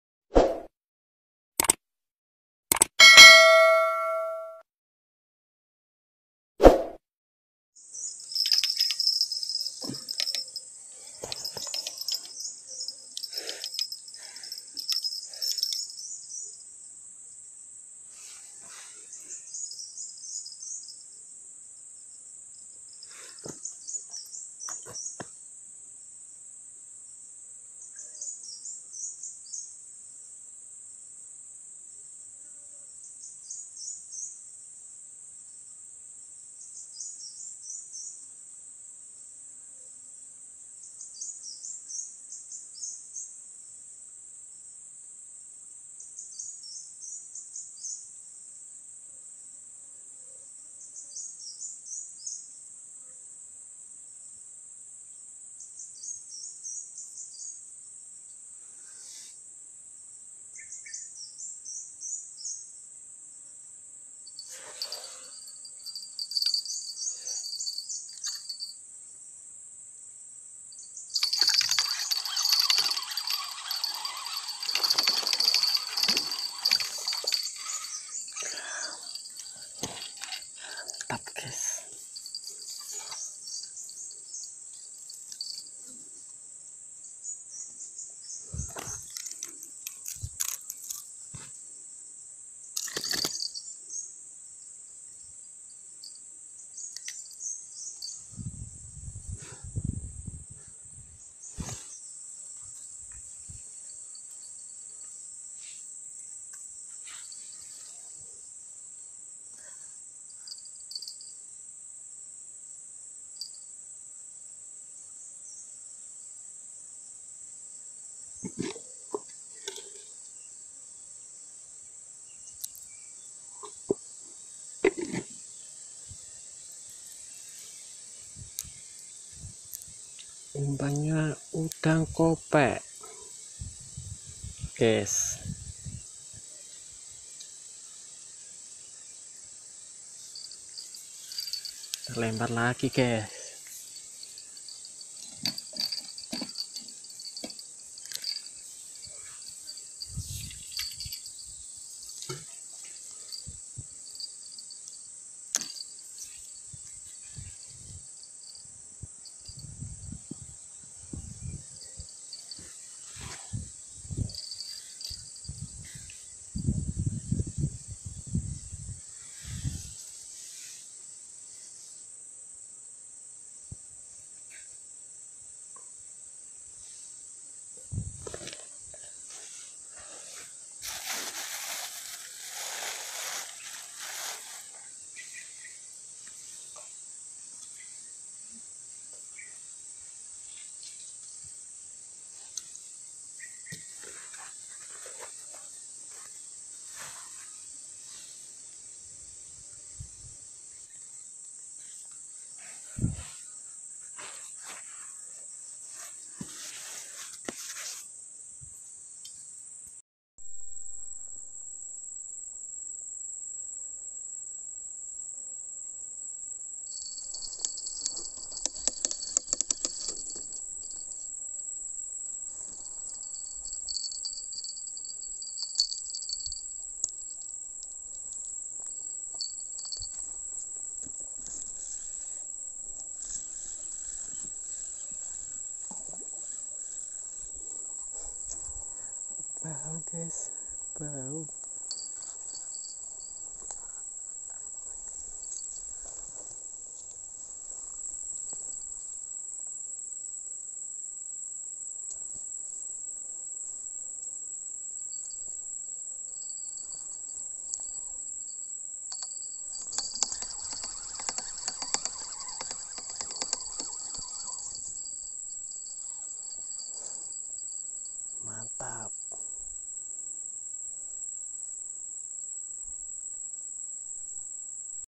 umpan udang kupas memang sesuai di segala medan..
umpan udang kupas memang sesuai di segala medan, sungai, laut,kolam